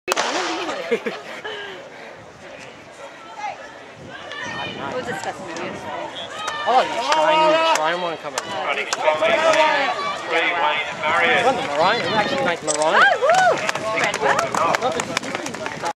100m Final Men's 55-59 National Masters Championships
Australian Masters Athletics Championships Hobart Saturday 8th March - Monday 10th March 2014